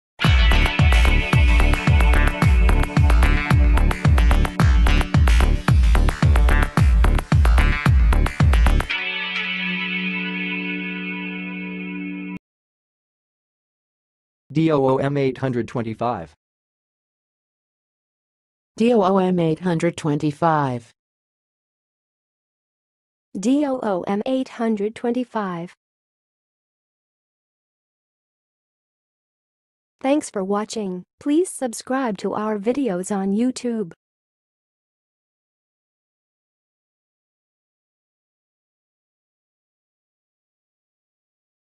How to say "doom825"! (High Quality Voices)
Watch in this video how to say and pronounce "doom825"!